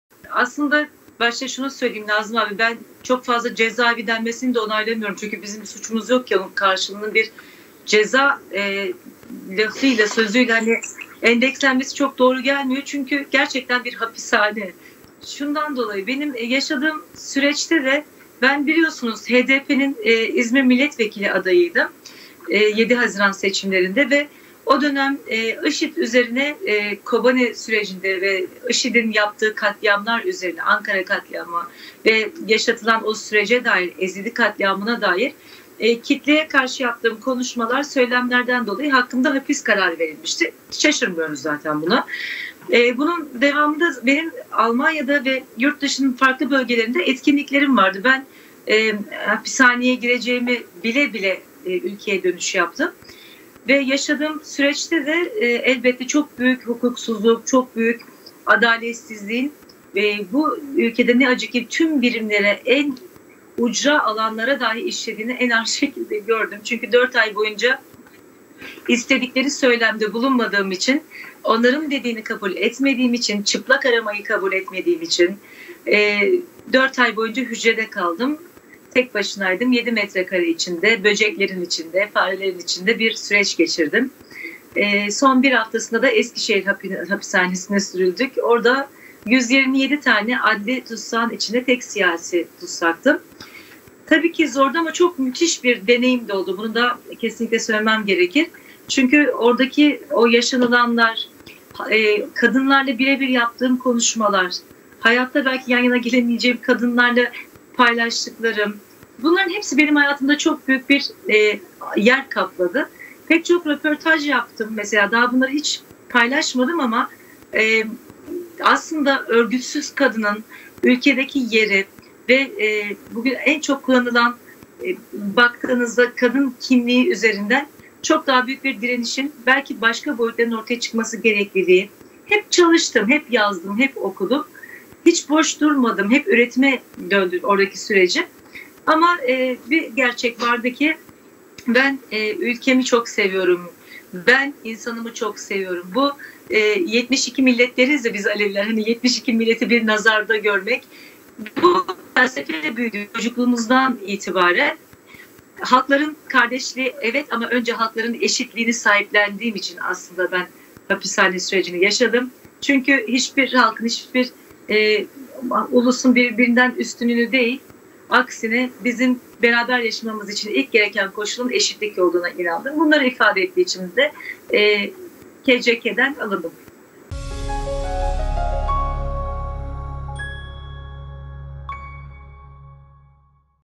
'IŞİD'in yaptığı katliamlara dair yaptığım konuşmalar nedeniyle hapse girdim'
Sanatçı Pınar Aydınlar, 'İstedikleri söylemde bulunmadığım için, onların dediğini kabul etmediğim için, çıplak aramayı kabul etmediğim için 4 ay boyunca hücrede kaldım' dedi.